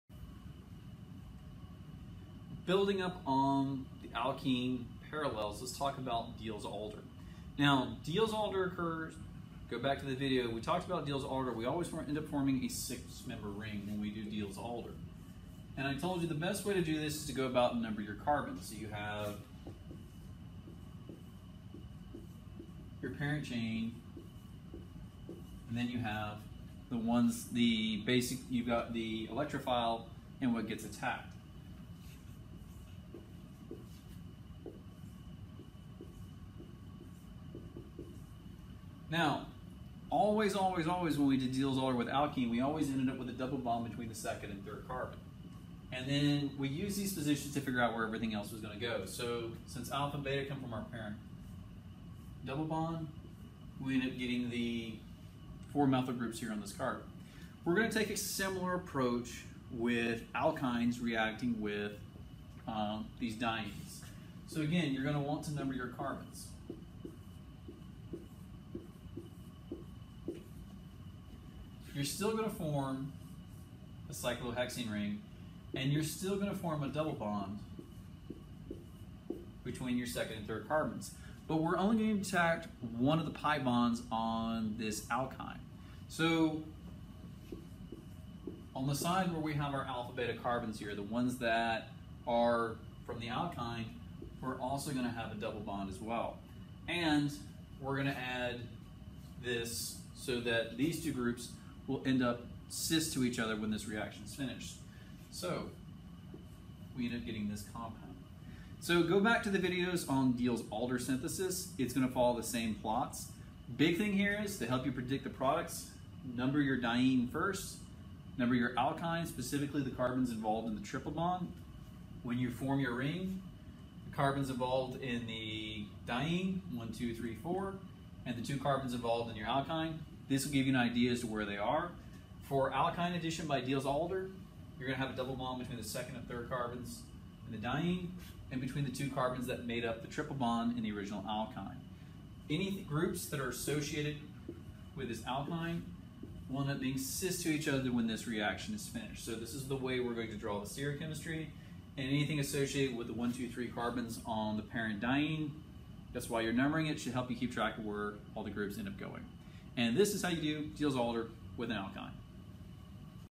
Diels Alder with Alkynes
Here we talk about how an alkyne behaves during Diels-Alder synthesis